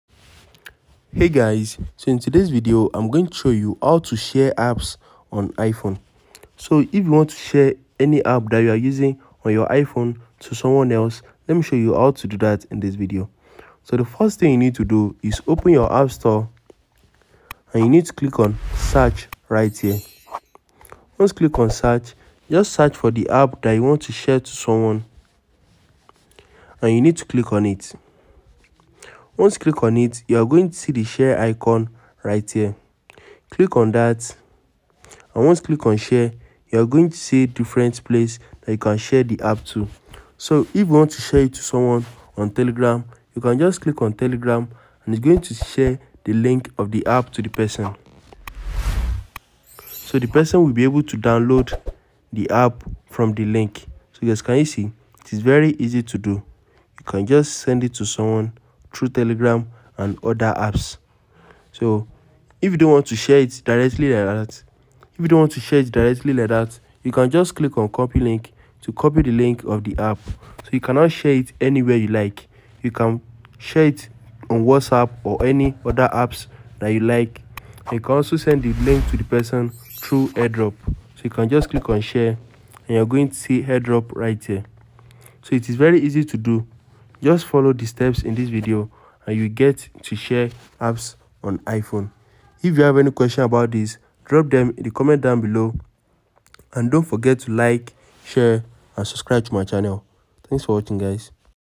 How to Share Apps on iPhone
Welcome to our iPhone tutorials! Sharing your favorite apps with friends and family has never been easier. In this video, we'll show you various methods to share apps on your iPhone, allowing you to spread the app love seamlessly.